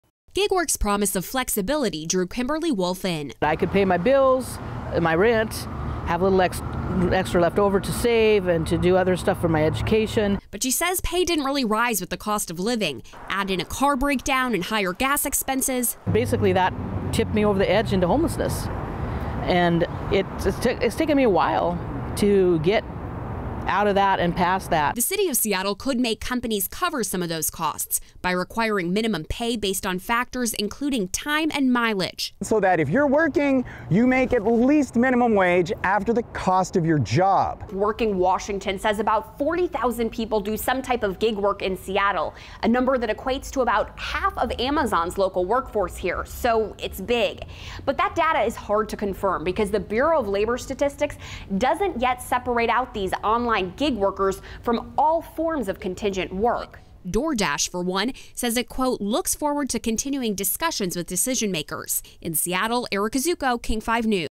Seattle City Council considering minimum pay for gig workers
The 'PayUp' proposal, as drafted, would require companies to provide minimum pay using a formula considering factors including time, mileage and a minimum wage rate.